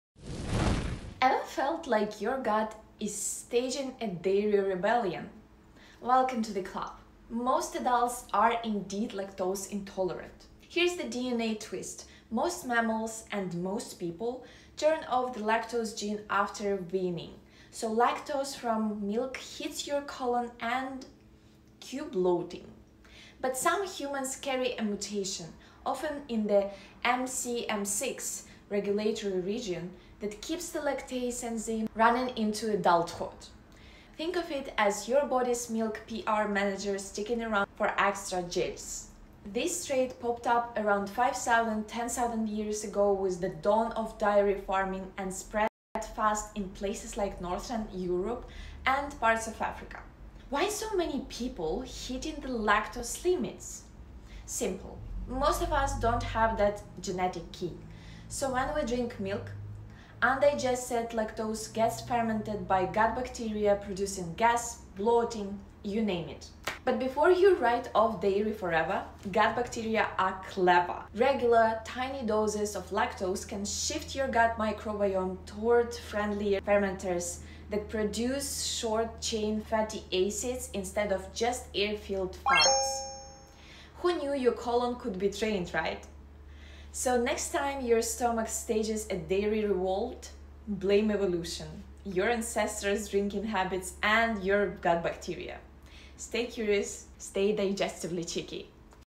Why Your Stomach Hates Milk 🥛 (Blame Evolution)
Why is everyone suddenly “lactose intolerant”? 🥛💥 Most humans actually are it’s our DNA’s default setting! Lactase persistence (the ability to keep digesting milk into adulthood) is a rare genetic twist that spread with dairy farming thousands of years ago. If milk makes your gut rebel, blame evolution, your ancestors’ diets, and your microbiome.